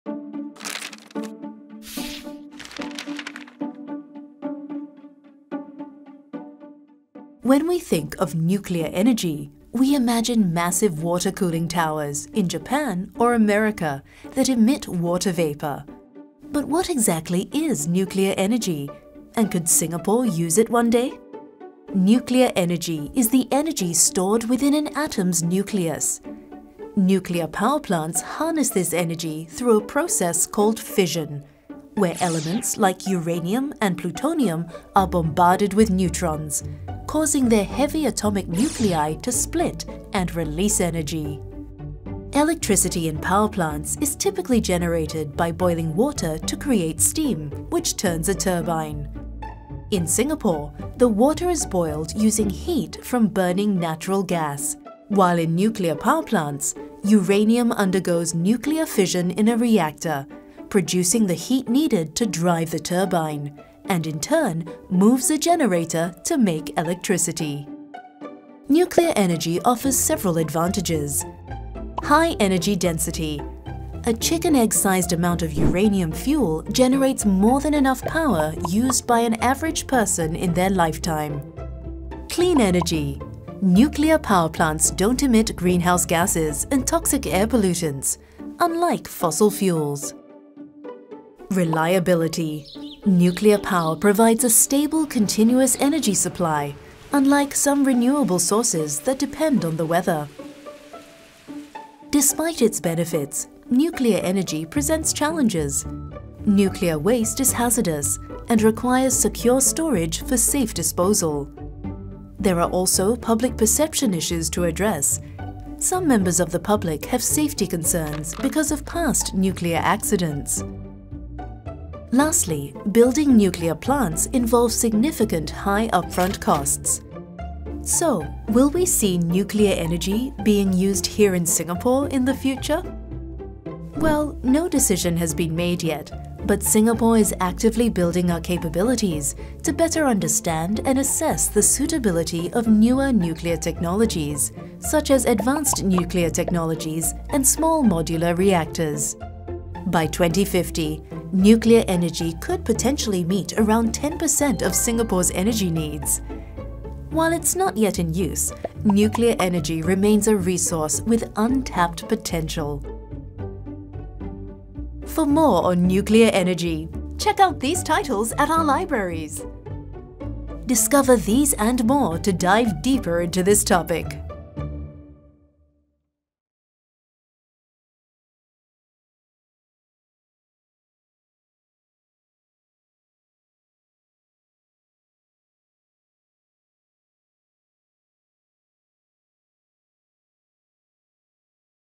Nuclear Energy | Explained in 3 Minutes
Singapore has been exploring the possibility of introducing nuclear energy into our energy mix with the use of Small Modular Reactors. But what is nuclear energy, and how can it be used in Singapore?

Join us as we offer bite-sized insights as to how nuclear energy can be generated in Singapore!

Timestamps:
00:00 – Opening title
00:21 – How heat is generated through nuclear fission
00:41 – How electricity is generated in power plants versus nuclear plants
01:10 – Advantages of nuclear energy
01:46 – Challenges of nuclear energy
02:11 – Future of nuclear energy in Singapore

Explained in 3 Minutes is part of NLB’s LearnX initiative, where you can learn about topics such as spanning from Digital, Sustainability, Wellness to Arts and more.